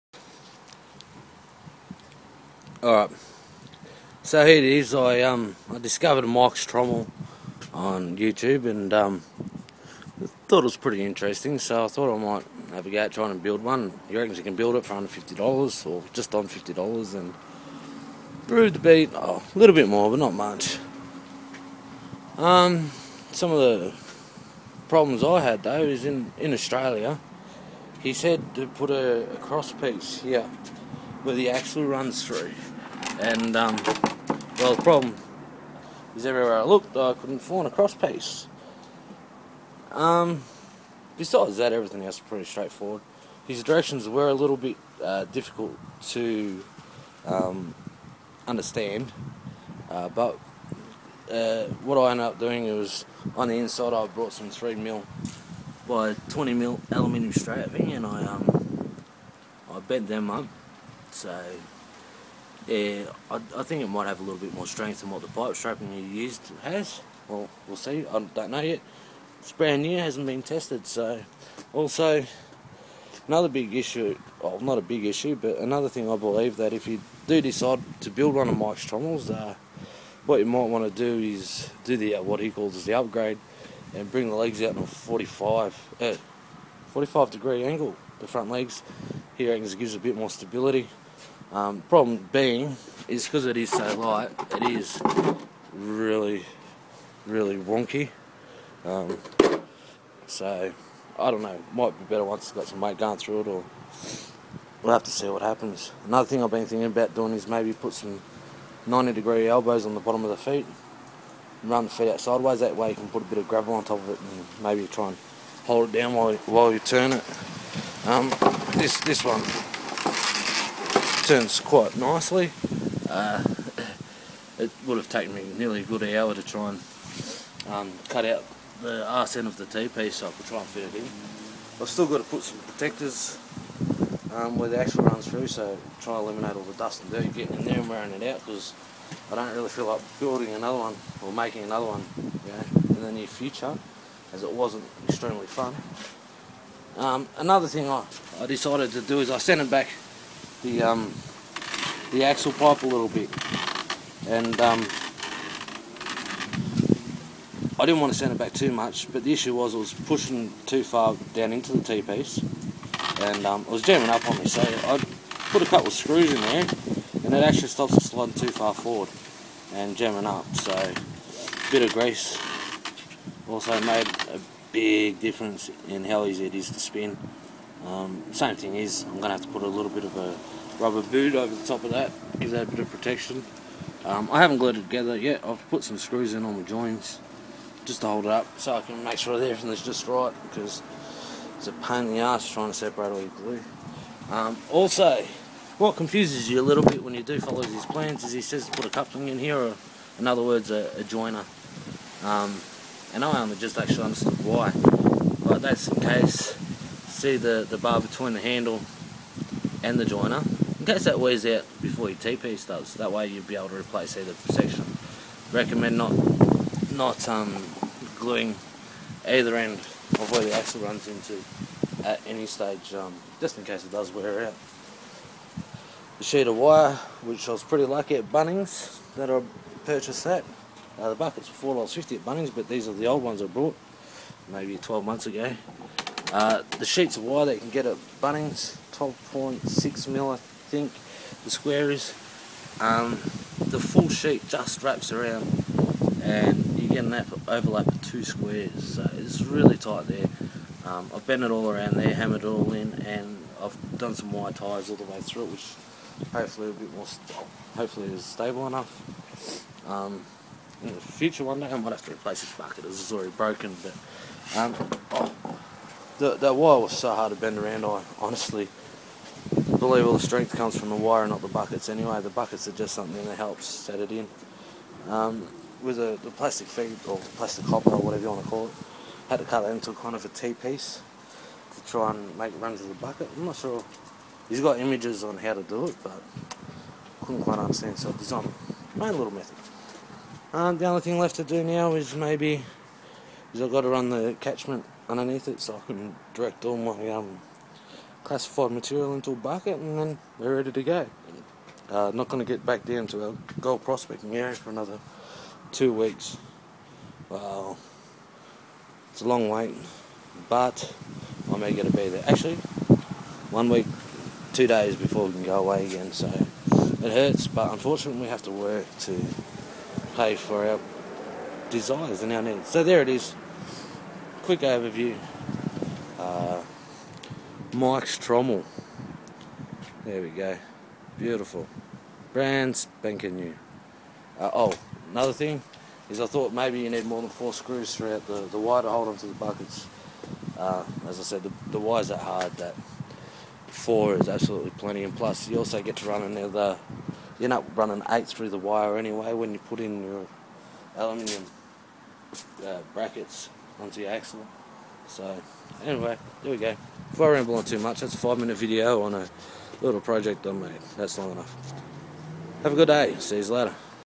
HOMEMADE TROMMEL CLASSIFIER SCREEN
ok so i made mikes trommel for when i go prospecting for gold, and this just shows an overveiw of the trommol and a few issues i had making it...